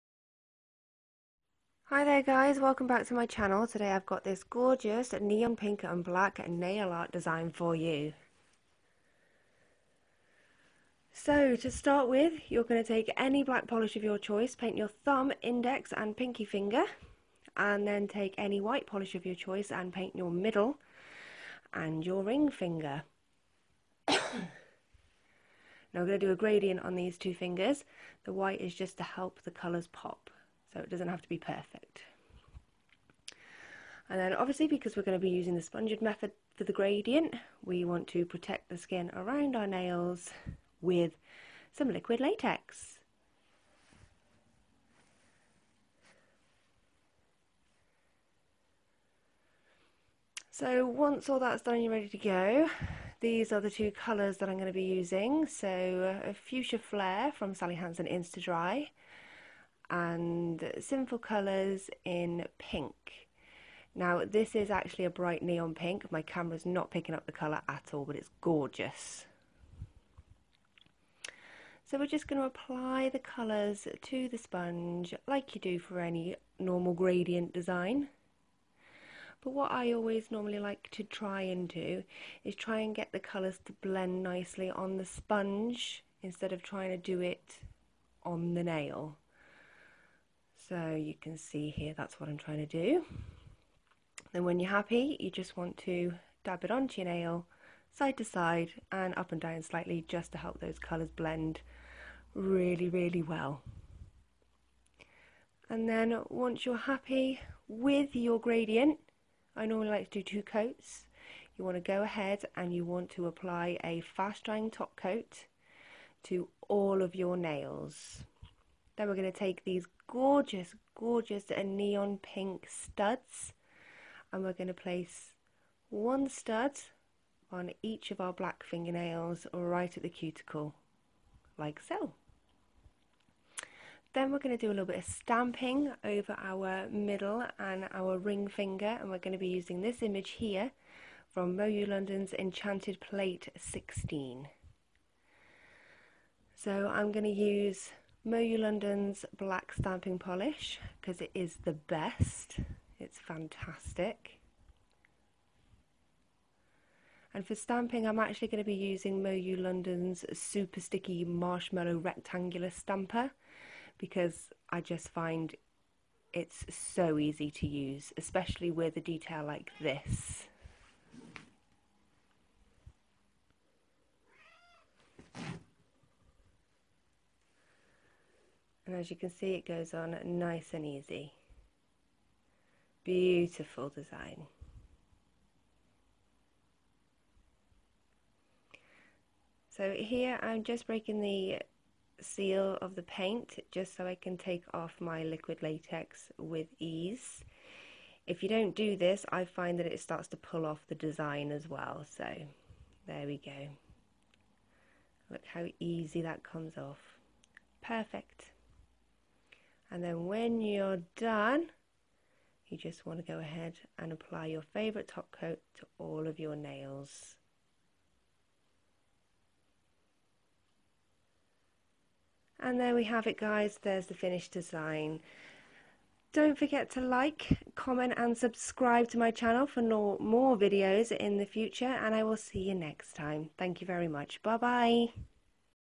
Nail art | Neon pink gradient with stamping
I love the colours in this nail art, they are great for everyday and perfect for a night out.
 Thanks all for watching, if you have any questions please feel free to leave them in the comments section below, don't forget to like and subscribe for more videos in the future. xxx
Below is the list of products I used:-
No7 Base coat
Makeup Gallery white polish
sinful colors PINK
Sally Hansen Insta Dry Fuchsia Flare
Moyou London Black polish (for painting and stamping)
Moyou London stamping plate enchanted 16
Moyou London rectangle sticky stamper
Born pretty store neon studs
Fast dry top coat